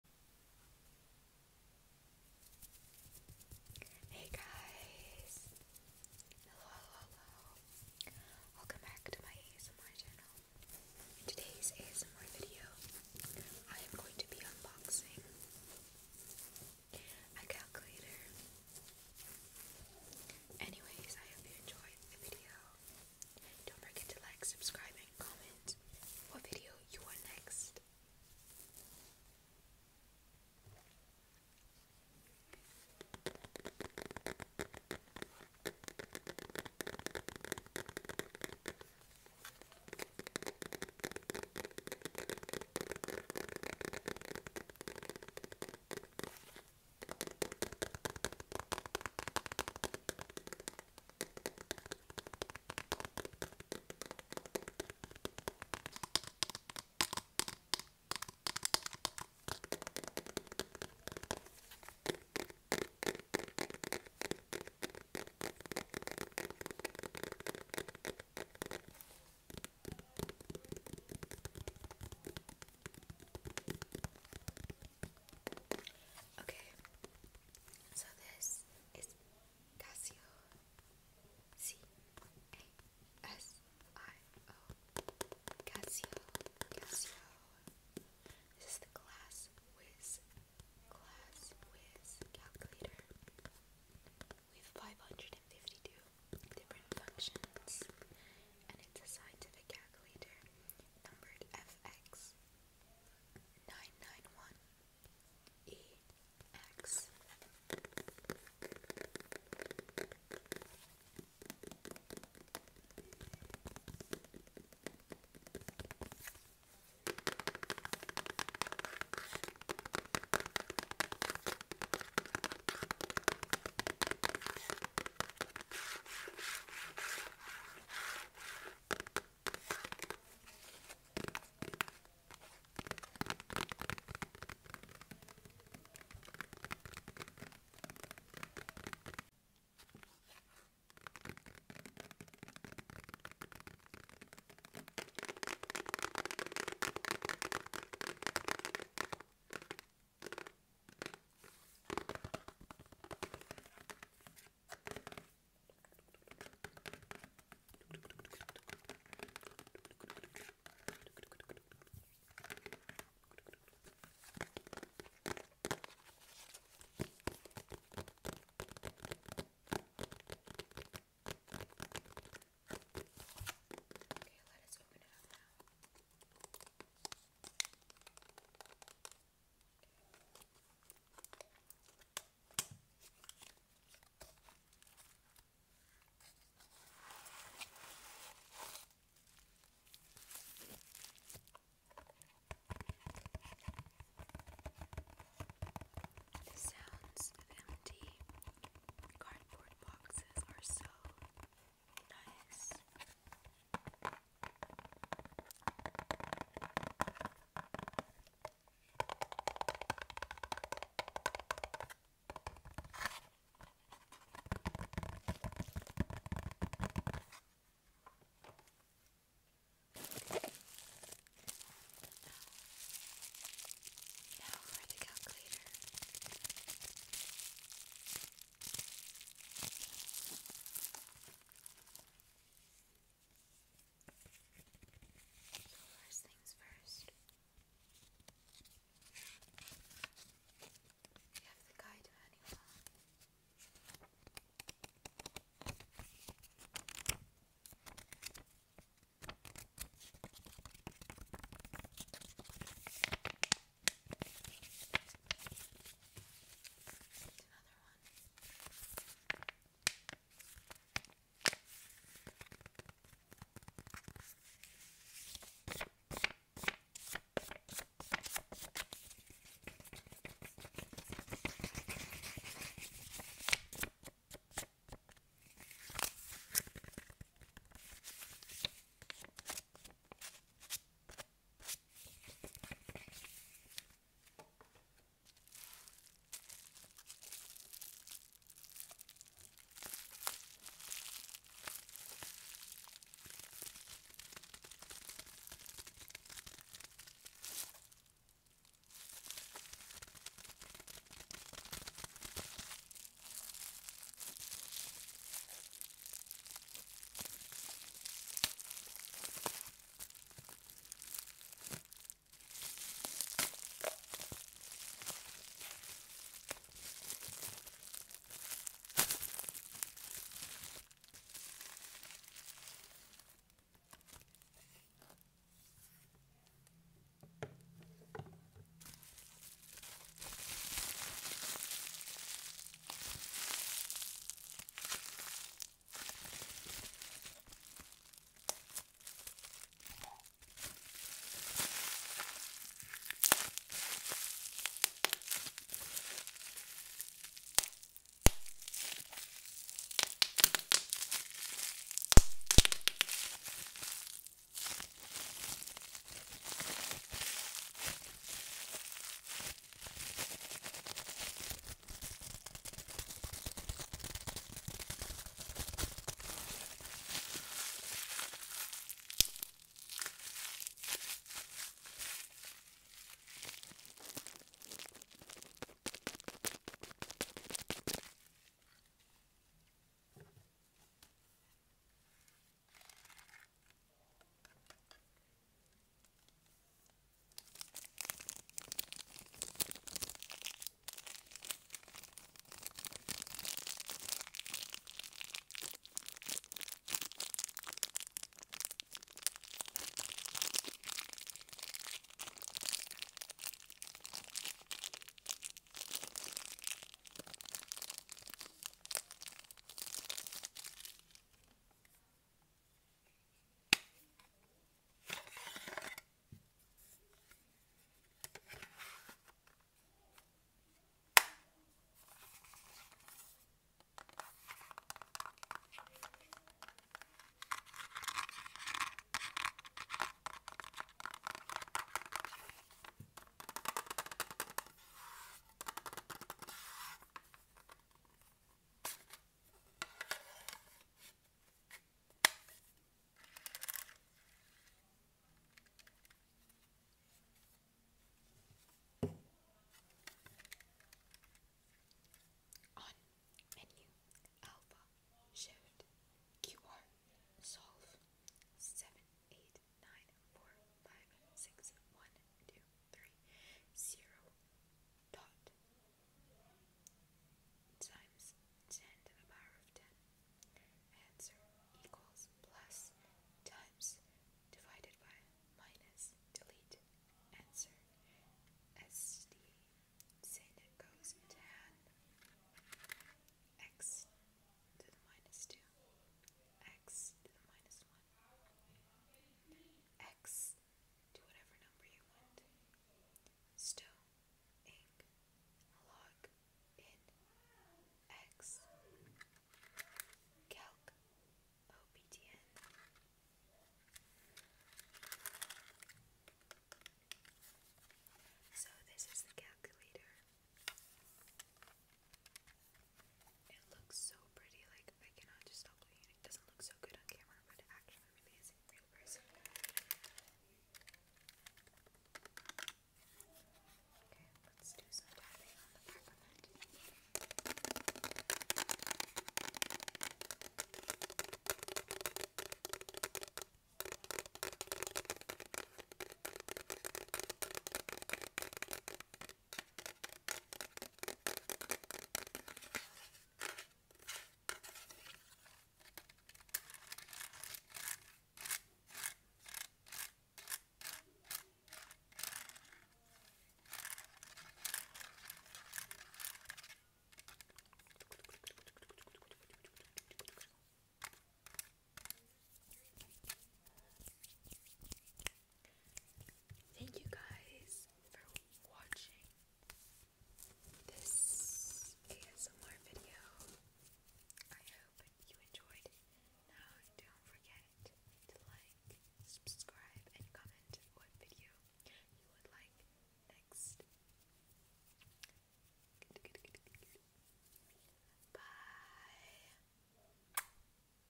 Asmr tapping, scratching and whispering on a calculator
This video is about Asmr tapping, scratching and whispering on a calculator